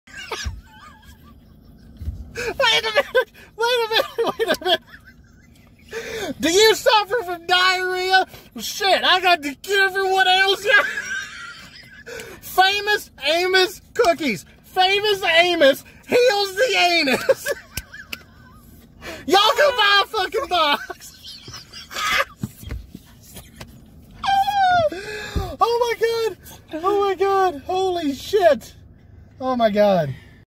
Carpool Comedy - Famous Amos Cookies
While Reagan and I aren't licensed physicians, you can still rely on us for sound medical advice! We hope you enjoy this edition of Carpool Comedy!